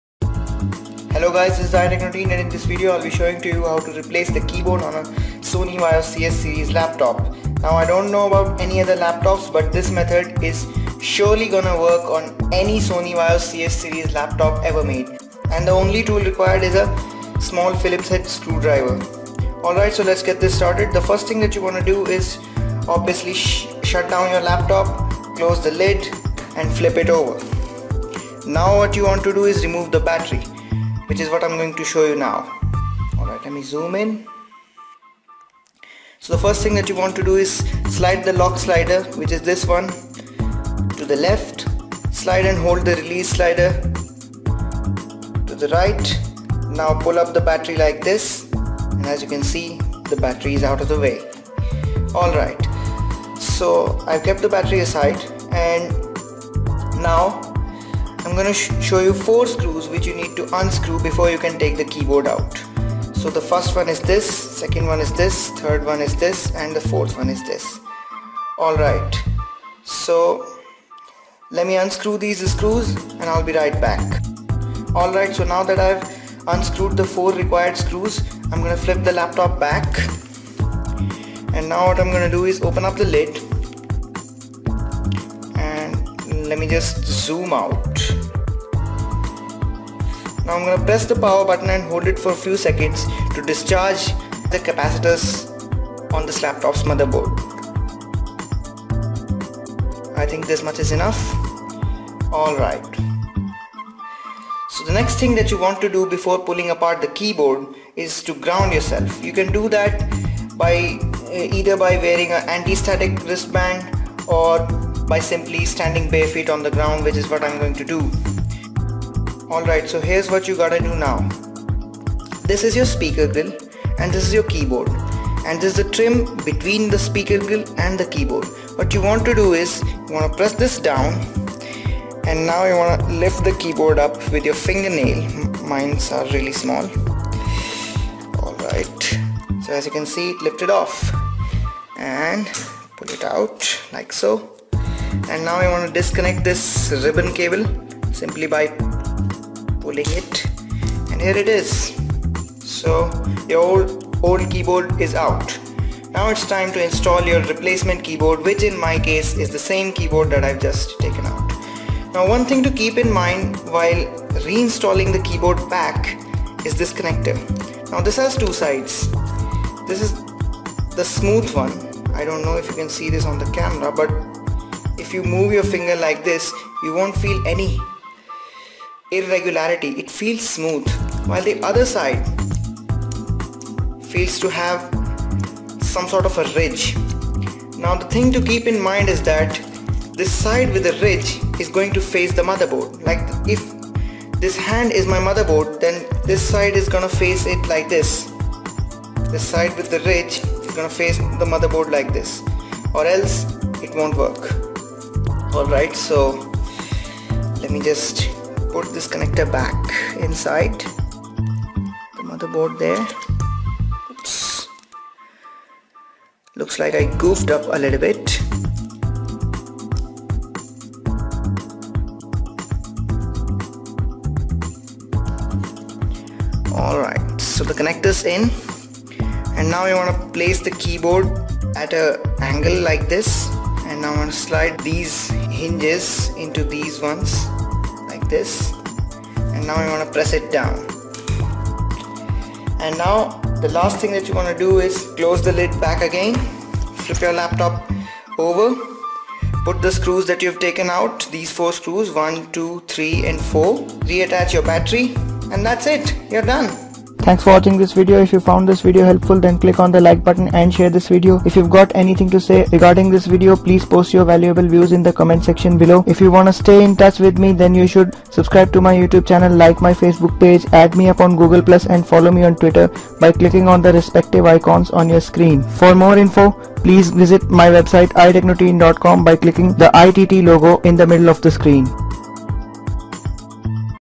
Sony Vaio keyboard replacement for CS Series laptops !
In this video I'll show you how you can replace the keyboard on any Sony Vaio CS Series laptop ever made !